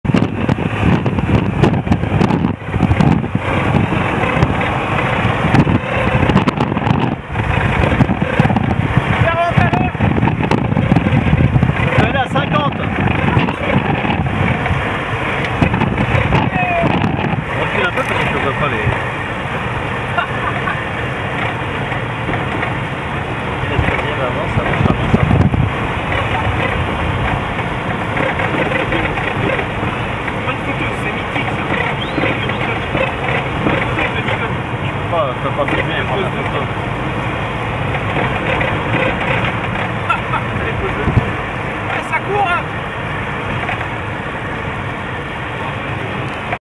Race With an Emu in The Australian Nullarbor Desert
Incredible moment when this emu started to run next to me in the Nullarbor desert in Australia during my World Tour on a bicycle (in 80 days by the way). It was in 2009. Unforgettable.

I am a sport journalist for international medias, author of 8 books and a cyclist with a passion for extreme challenges.